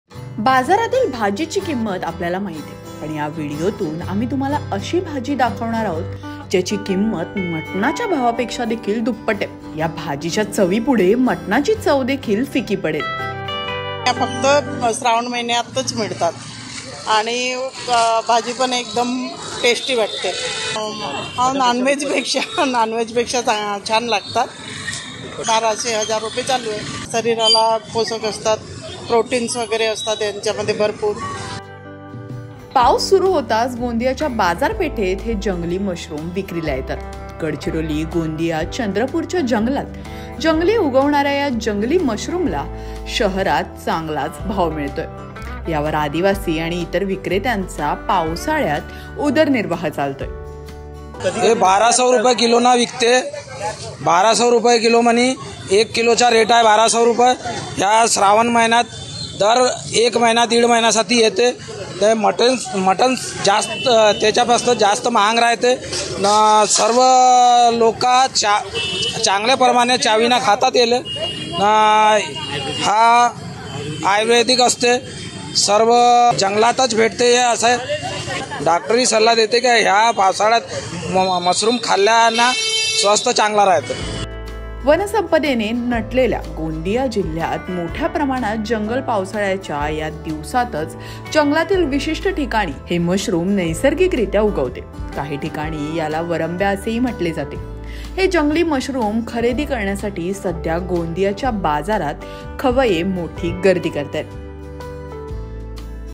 या जंगली मशरूमची किंमत तर पहा ?...| Expensive Junglee Mushroom | MaxMaharashtra
मटणाच्या जेवणाला फिके पाडणाऱ्या या जंगली मशरूमचा भाव ऐकून अवाक व्हाल. केवळ श्रावण महिन्यातच फुलणारे हे जंगली मशरूम खायला लोकांची होतेय गर्दी.